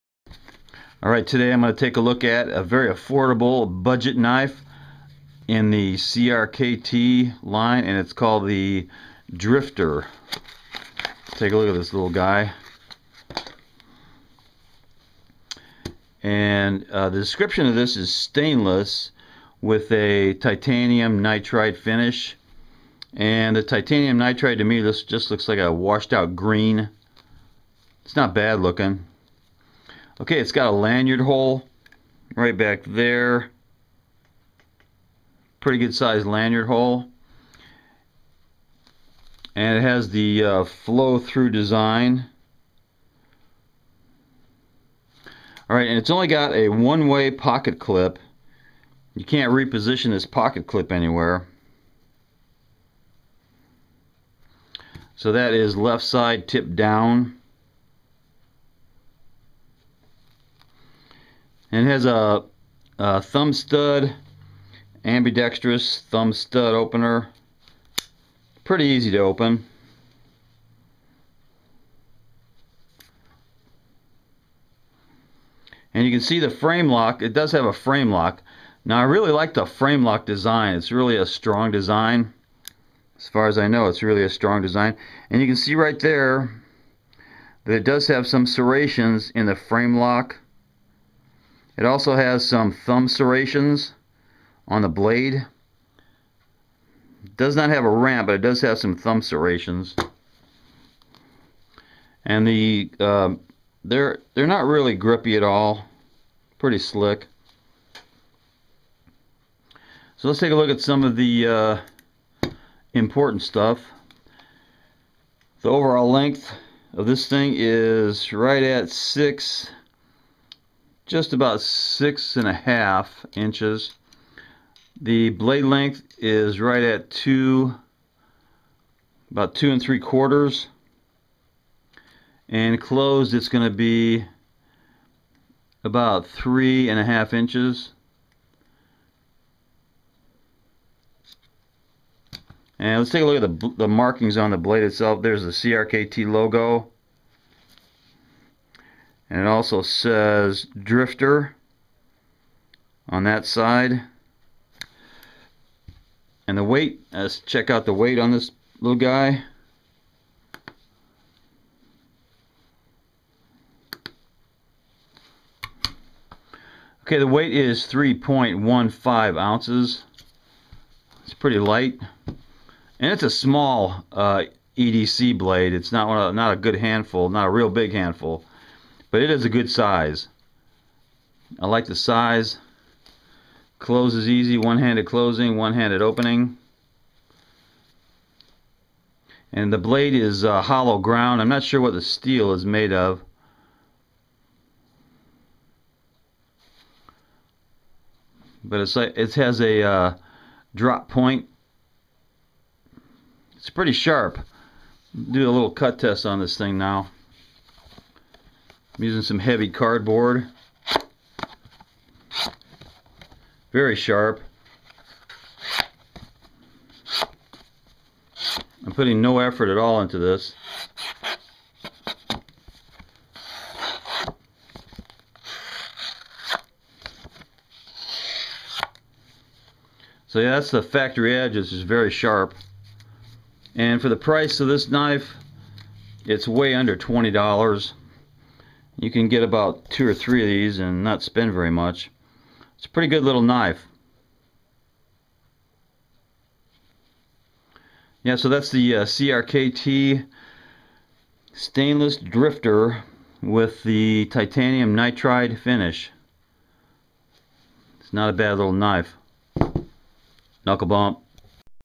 CRKT's Drifter
For way under $20 it's a deal.
The 8CR14mov stainless blade is VV sharp.